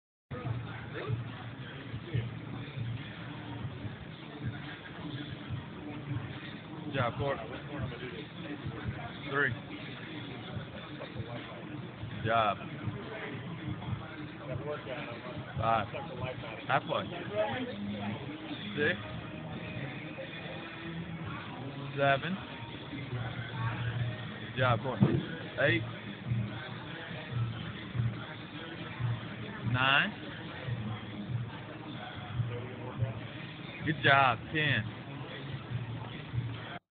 150lb Back Squat
Courtney hitting 150lb back squat for 3 sets of 10!